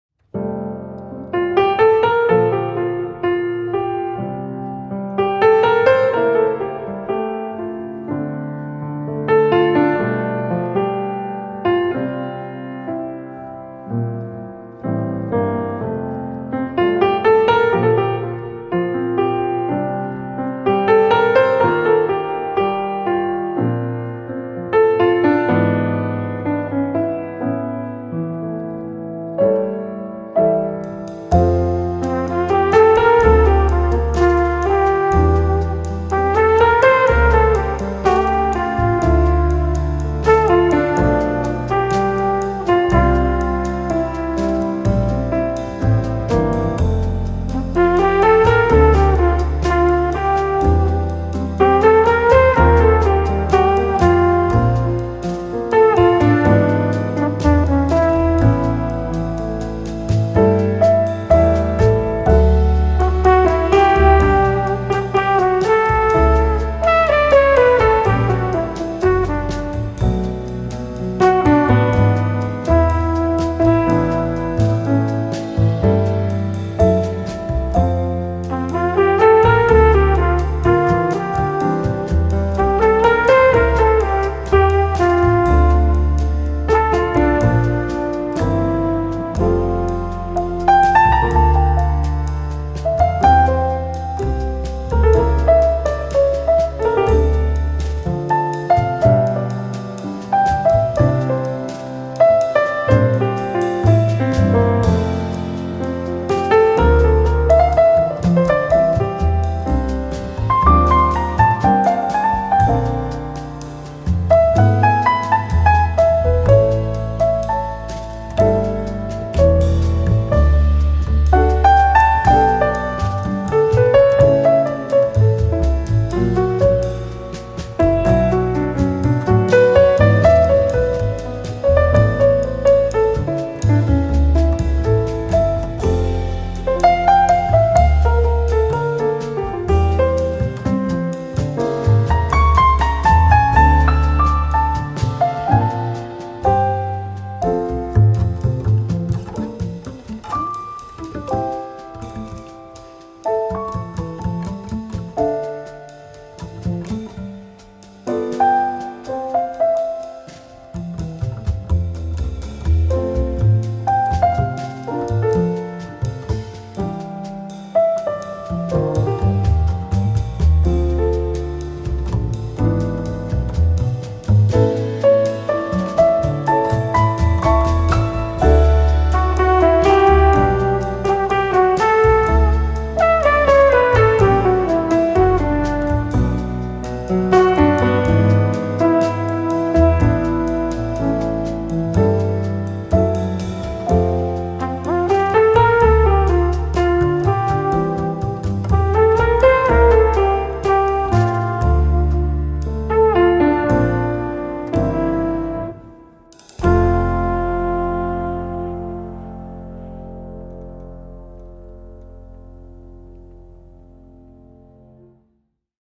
[Easy ABSTRACT PAINTING with BEAUTIFUL SMOOTH JAZZ] PAINTING IDEAS with JAZZ MUSIC #4
Enjoy Easy Abstract Paintings with Beautiful Smooth Jazz. Abstract Painting Ideas. Paint with Music. NO ADS

Stay Safe, Healthy, Relaxed and Happy